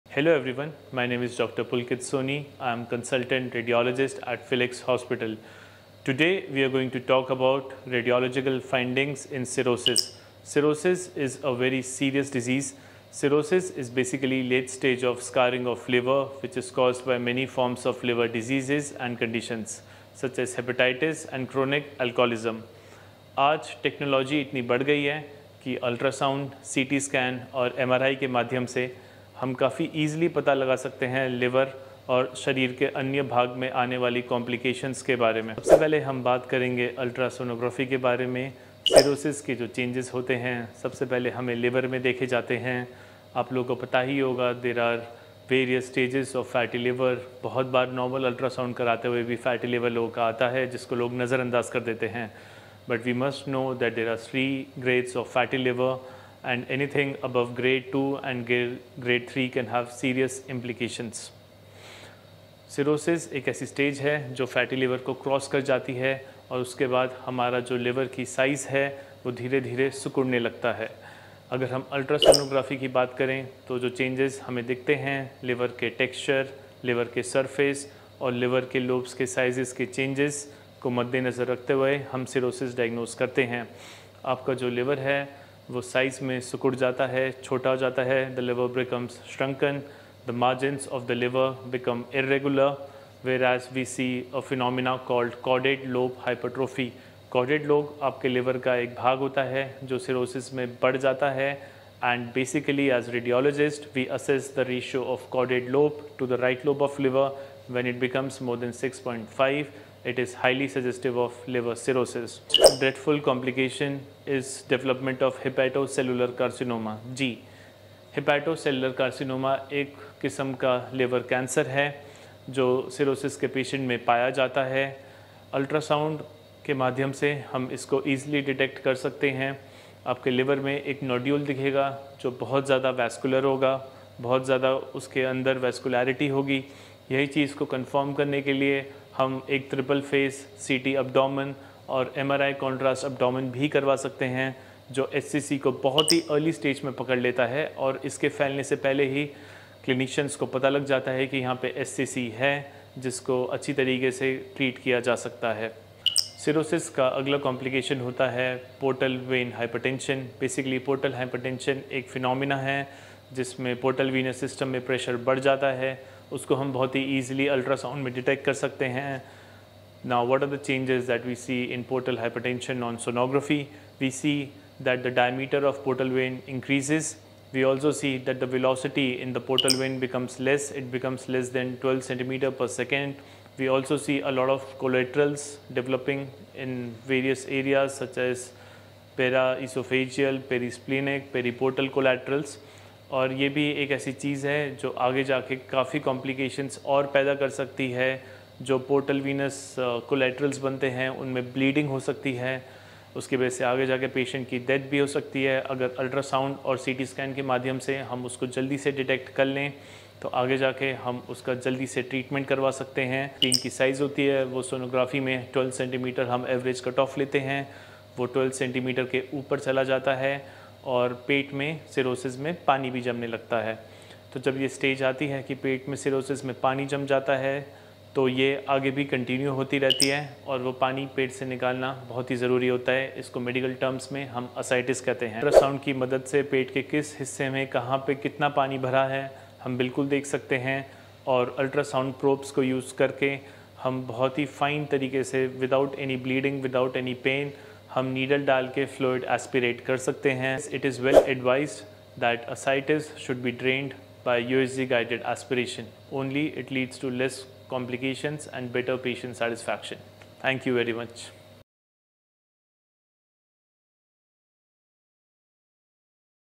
Why is Liver Cirrhosis such a Silent Threat? 🤔
Join us for an eye-opening journey into understanding liver cirrhosis, where Dr. Pulkit Soni breaks down complex medical terminology into simple, digestible nuggets of knowledge. Learn about its causes, symptoms, and treatment options in this enlightening video. Don't miss out – your liver health matters!